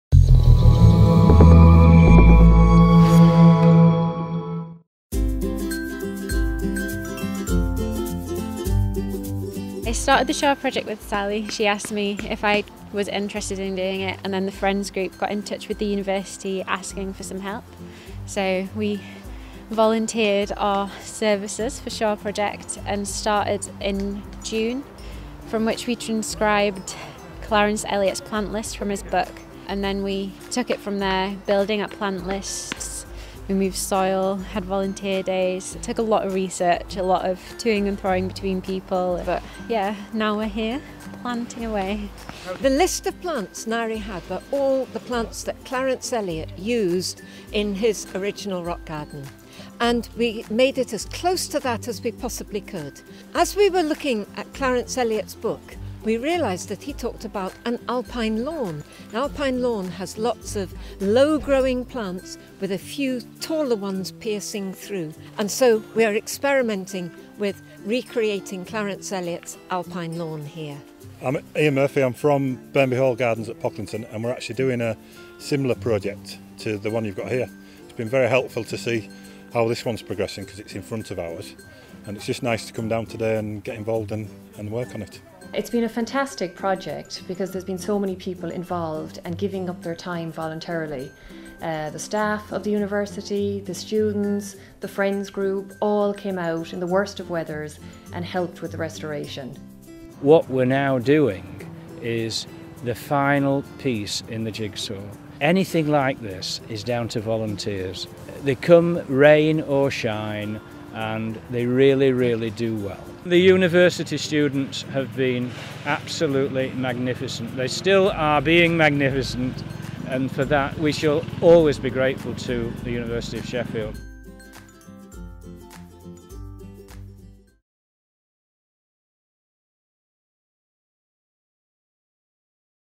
Restoration of Whinfell Quarry Gardens
In November 2015, undergraduate Landscape Architecture student Ngaire Burston helped to restore 'little quarry' within Whinfell Quarry Gardens in Sheffield, as part of her SURE (Sheffield Undergraduate Research Experience) project.